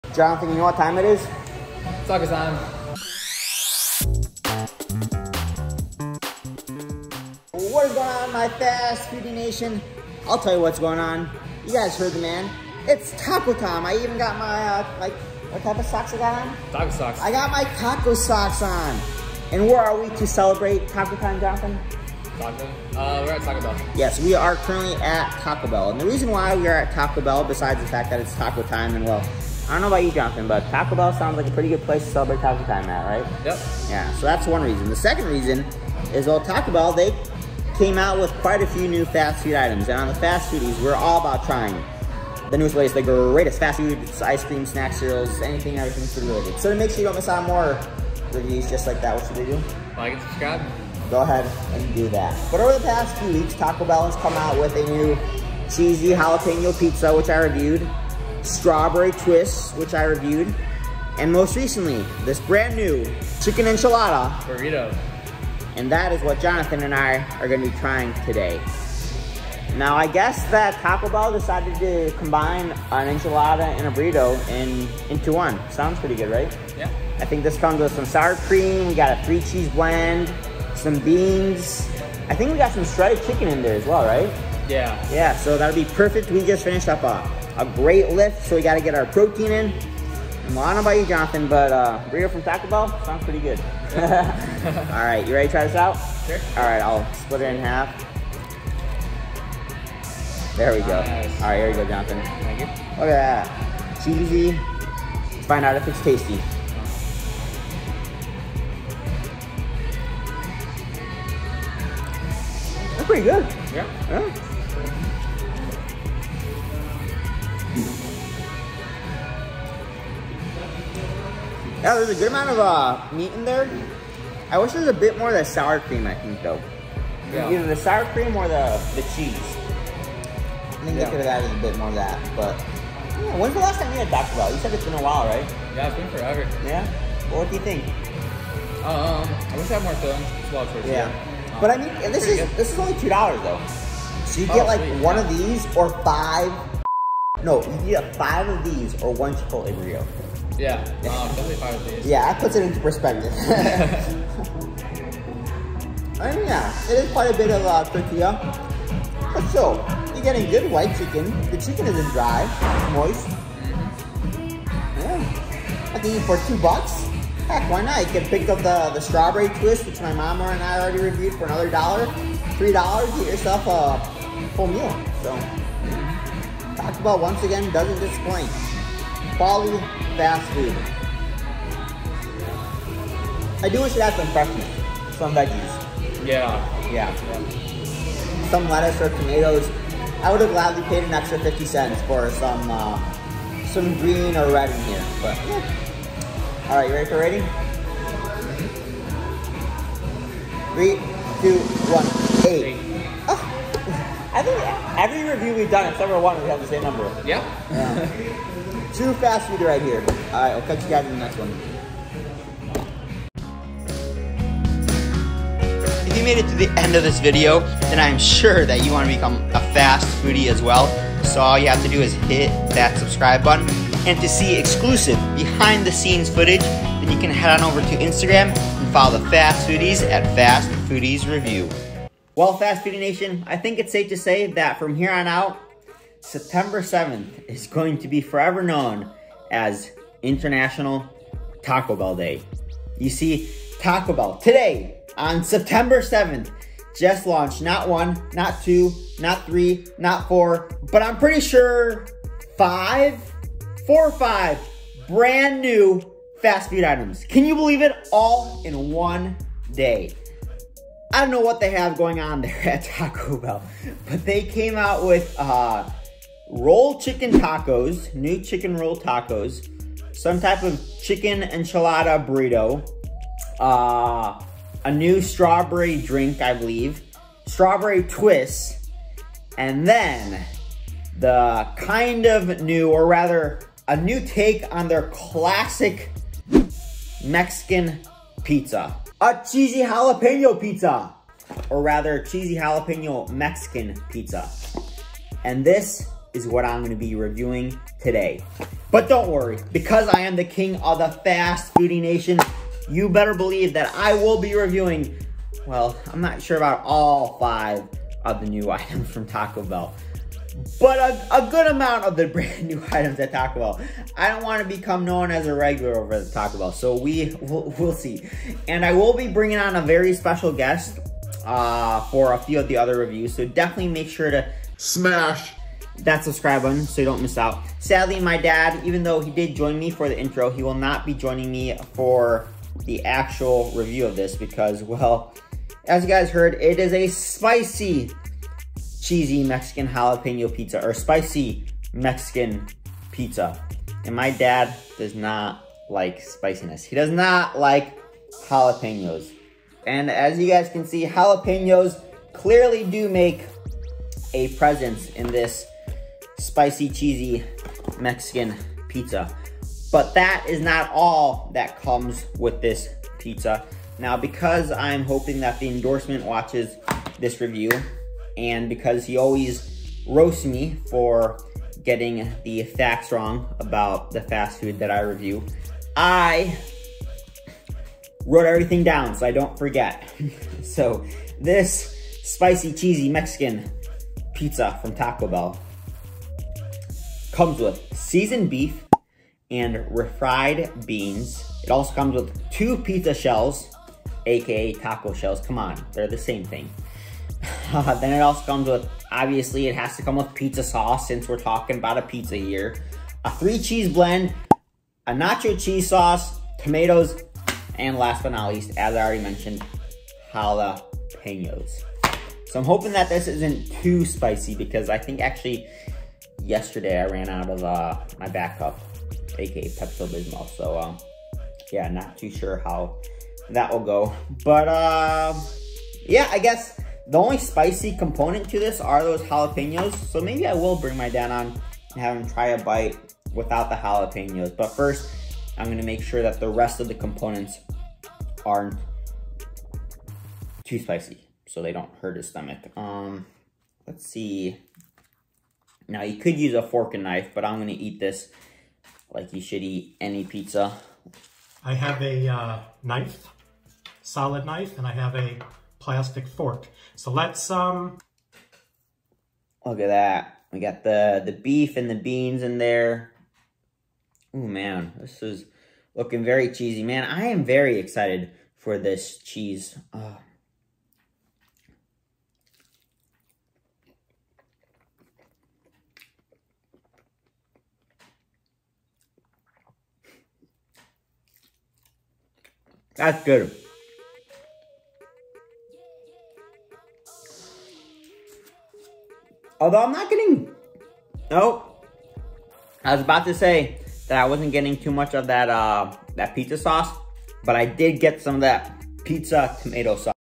Taco Bell's New Chicken Enchilada Burrito Review 🌯 | Must-Try Fast Food Taste Test
Greetings, foodies! Welcome back to Fast Foodies, the ultimate destination for fast food reviews and culinary adventures. Today, we're sinking our teeth into something exciting – Taco Bell's brand-new Chicken Enchilada Burrito. 🌯🔥

In this video, I'll give you the lowdown on this Tex-Mex sensation, sharing my honest thoughts on flavor, spice, and overall satisfaction. Is it a crave-worthy addition to Taco Bell's menu or a miss? Join me as we unravel the delicious details.

As we unwrap this burrito together, you'll get an up-close look at the hearty ingredients that make up this tempting creation. From the tender chicken to the zesty enchilada sauce, we're leaving no bite unexplored.

If you're a fan of Mexican-inspired fast food or simply enjoy living life one burrito at a time, this review is a must-watch. Let's taco 'bout it! 🌮🤤🎉

Parker-kerth
or